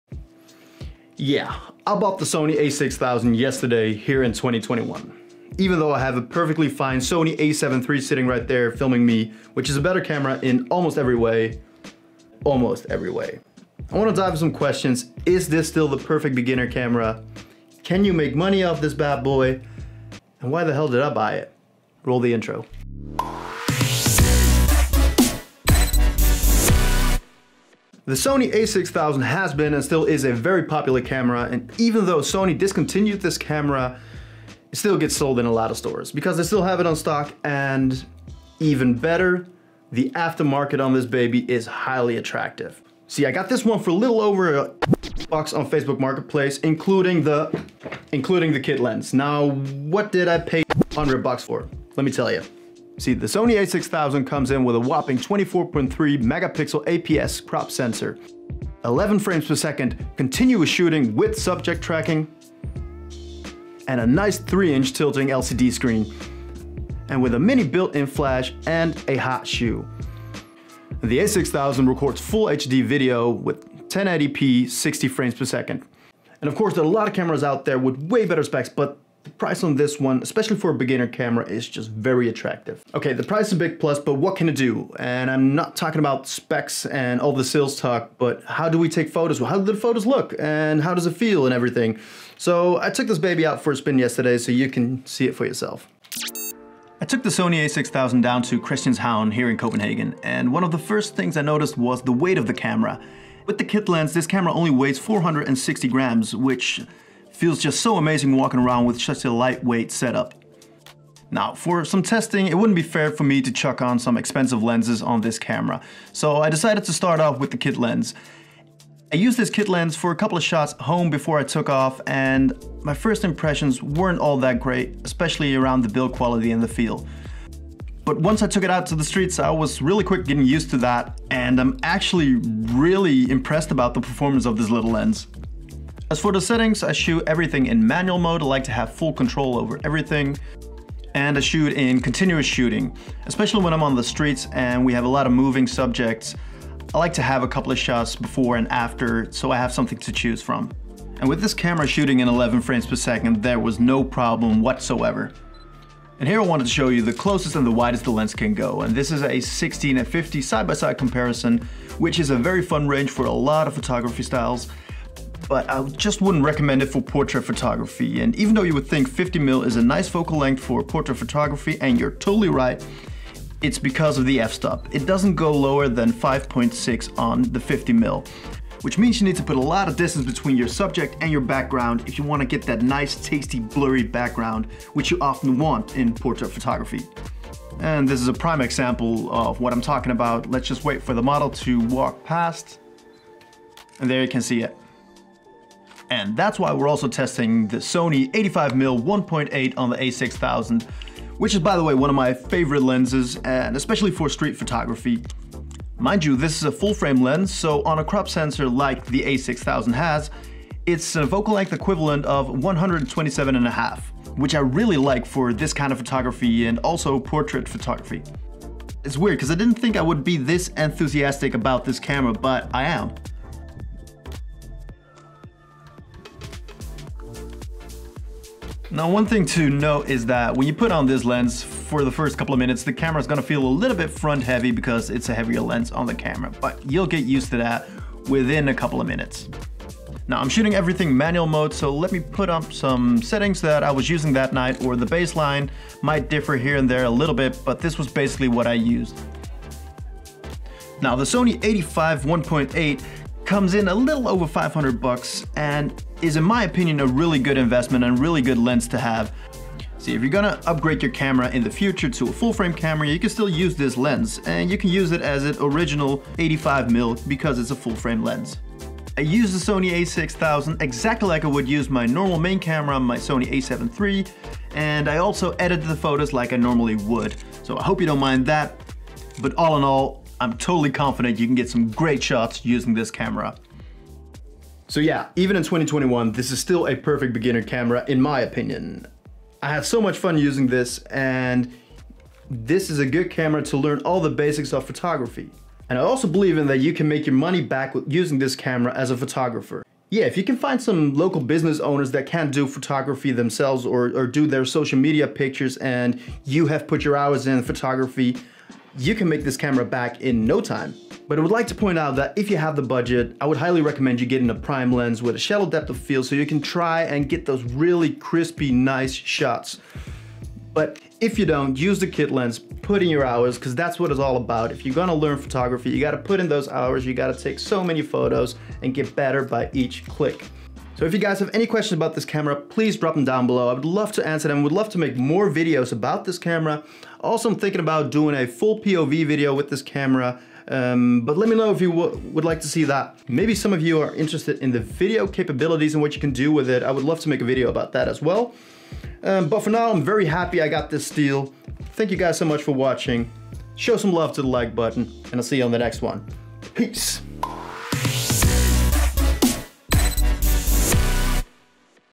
BEST BEGINNER CAMERA for Photography 2021 - Still the SONY A6000?!
Is the sony a6000 still the best beginner camera for photography in 2021 or one of the best beginner camera's for photo's in 2021? *The Best Camera for beginners is always hard to choose, especially on a budget. So I bought the sony a6000 in 2021 to show you if it's still a good camera for you.

CHAPTERS:
0:00 Intro
0:27 A6000 Best Price & Where
0:52 A6000 Specs
1:36 Taking Photo's with the Sony A6000 + Kit lens (POV)
3:17 A6000 with the Sony 85mm 1.8 (POV)
4:44 A6000 Best beginner camera in 2021?
5:32 ASK ME & Outro

Thanks!